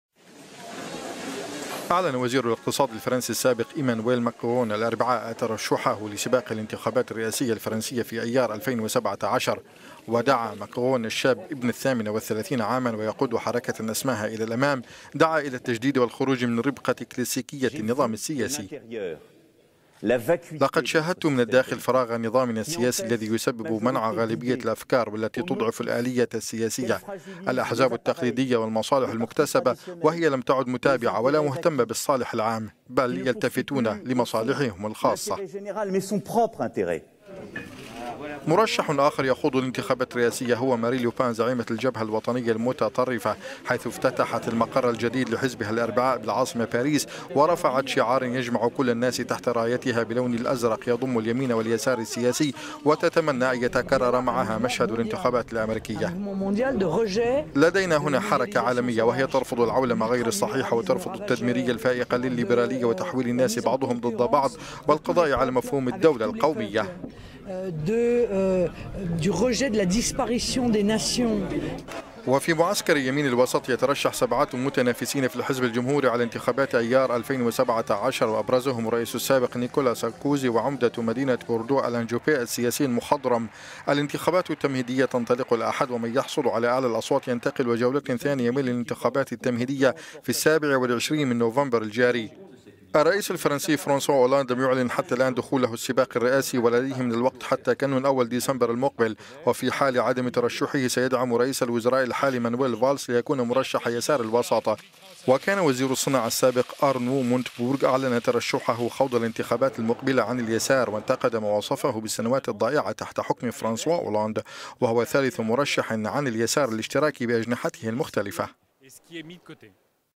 ماكرون يسابق للإليزيه ويضعف معسكر اليسار الاشتراكي الحاكم
بعد طول انتظار اعلن وزير الاقتصاد الفرنسي السابق ايمانويل ماكرون الاربعاء ترشحه لسباق الانتخابات الرئاسية الفرنسية في ايار 2017، ويدعو ماكرون الشاب ابن الثامنة والثلاثين عاما ويقود حركة اسماها "الى الامام" الى التجديد والخروج من ربقة كلاسيكية النظام السياسي، ترشح قد يفجر اليسار الاشتراكي ويضعف حظوظه.
" لقد شاهدت من الداخل فراغ نظامنا السياسي الذي يسبب منع غالبية الافكار والتي تضعف الآلية السياسية، الاحزاب التقليدية والمصالح المكتسبة، وهي لم تعد متابعة ولا مهتمة بالصالح العام، بل تلتفت لمصلحتها…